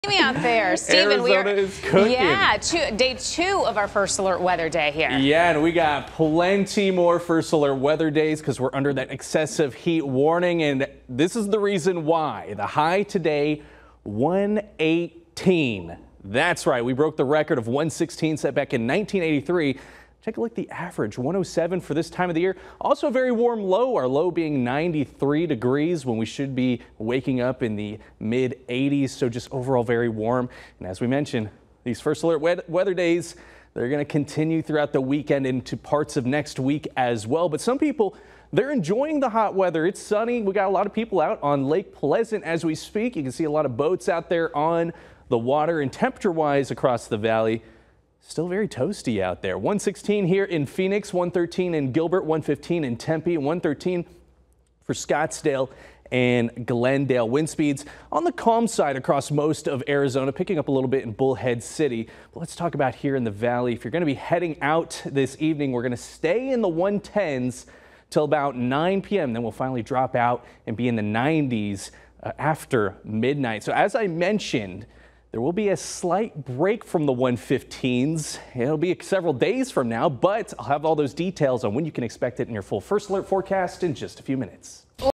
Heatwave hell: Phoenix high temp soars to 118º
In Phoenix, Sky Harbor Airport reached 118 degrees by 3 p.m. Friday, July 5th, breaking the record daily high of 116º set in 1983.